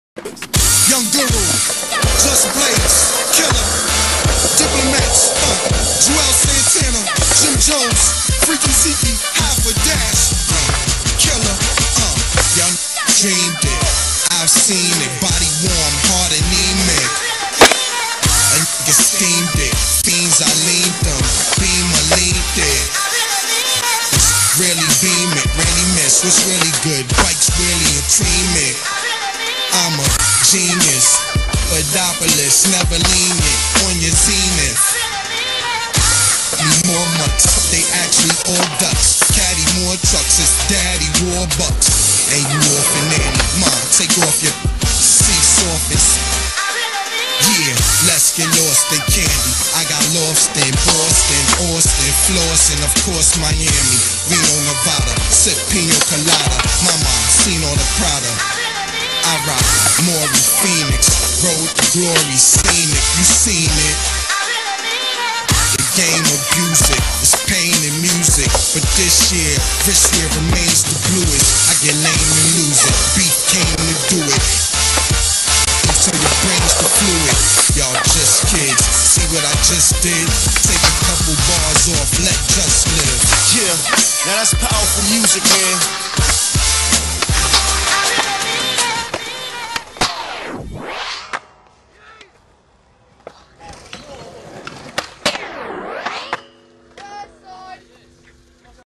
Tauranga 2
would have more footy but the tape gave out on me and fricked up a few tricks and was just testing out the ramps at the end. and yes its not the best filming